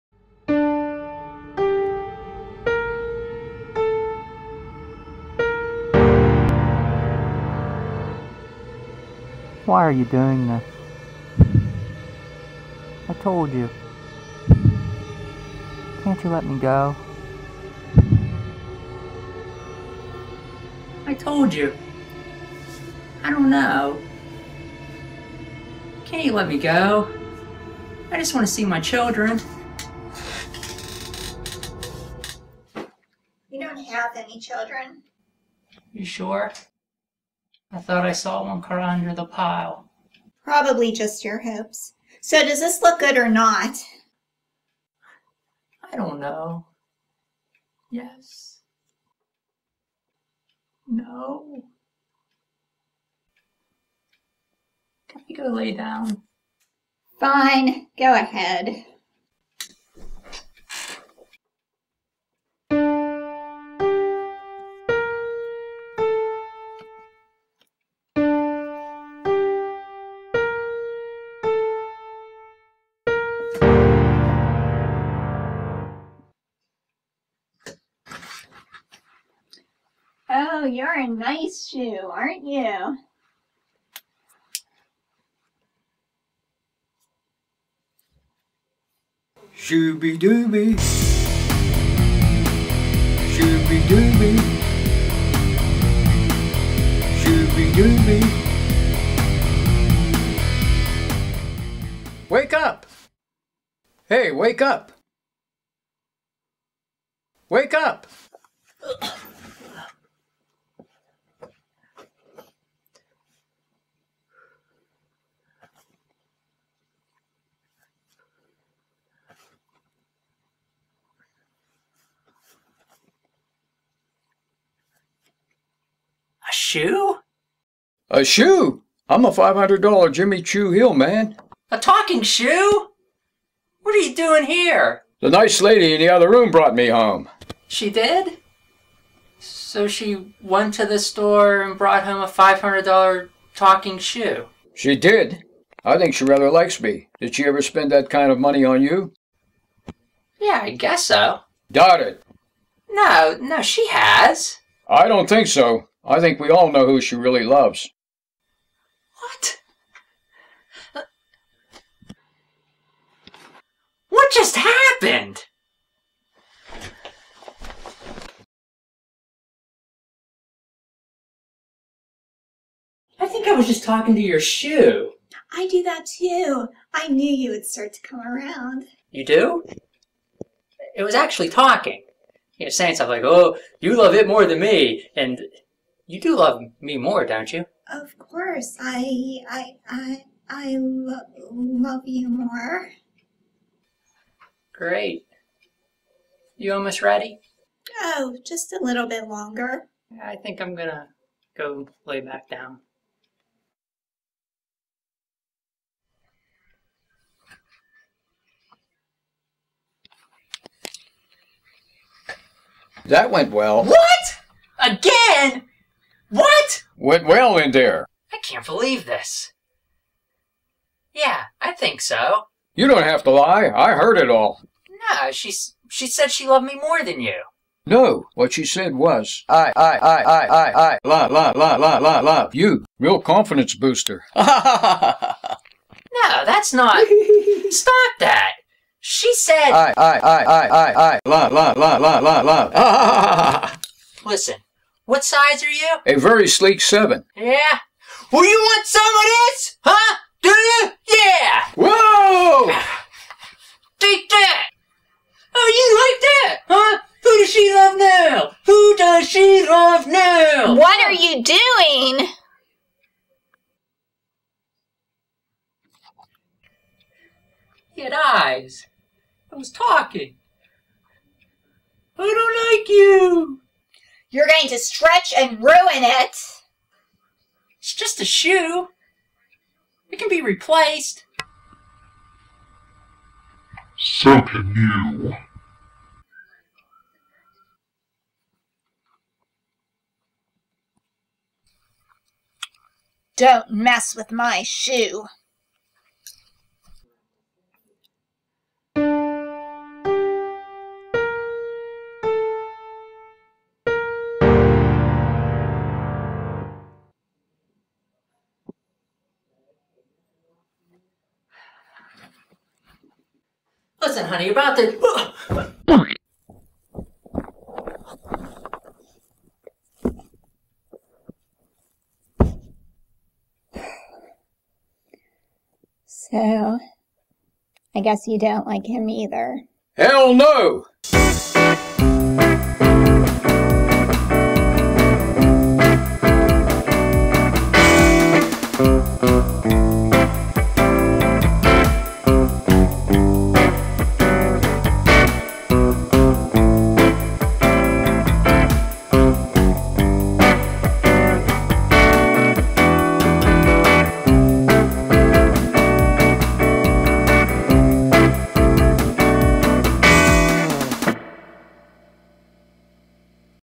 Pair - Tragic Fashion Love Story
Your classic love triangle: man, woman, shoe.  Who does she love more?  I think we all know.

Done for Video Prod II class.